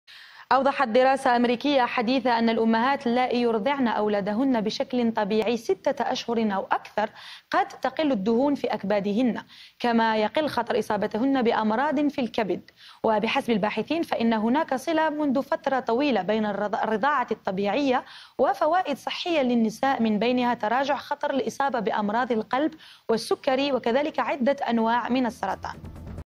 دراسة: الرضاعة الطبيعية تحمي الكبد
أوضحت دراسة أمريكية حديثة أن الأمهات اللائي يرضعن أولادهن بشكل طبيعي ستة أشهر أو أكثر قد تقل الدهون في أكبادهن كما يقل خطر إصابتهن بأمراض في الكبد.

وبحسب الباحثين فإن هناك صلة منذ فترة طويلة بين الرضاعة الطبيعية وفوائد صحية للنساء من بينها تراجع خطر الإصابة بأمراض القلب والسكري وأنواع معينة من السرطان.

وركزت الدراسة الحالية على ما إذا كانت الرضاعة مرتبطة أيضا بتراجع خطر الإصابة بمرض الكبد الدهني غير الكحولي الذي عادة ما يكون مرتبطا بالبدانة وعادات أكل معينة.

وتابع الباحثون 844 امرأة لمدة 25 عاما بعد وضعهن. وبشكل عام أبلغت 32 في المئة منهن إرضاعهن أولادهن لفترة شهر و25 في المئة قلن إنهن أرضعن أولادهن بشكل طبيعي لفترة تراوحت بين شهر وستة أشهر و43 في المئة ذكرن إرضاعهن أولادهن لفترة أطول.

وذكر الباحثون في دورية أمراض الدم أنه بحلول نهاية الدراسة كان عمر هؤلاء النسوة 49 عاما في المتوسط. وأصيبت 54 امرأة أو ستة في المئة منهن بالكبد الدهني غير الكحولي. وقل احتمال إصابة النساء اللائي أرضعن أولادهن ستة أشهر على الأقل بأمراض في الكبد بنسبة 52 في المئة بالمقارنة مع الأمهات اللائي أرضعن أولادهن أقل من شهر .

وقالت الطبيبة فيرال أجميرا من جامعة كاليفورنيا في سان دييجو إن "هذا التحليل الجديد يسهم في الكم المتزايد من الأدلة التي تثبت أن رضاعة الطفل بشكل طبيعي تمثل أيضا فوائد صحية مهمة للأم".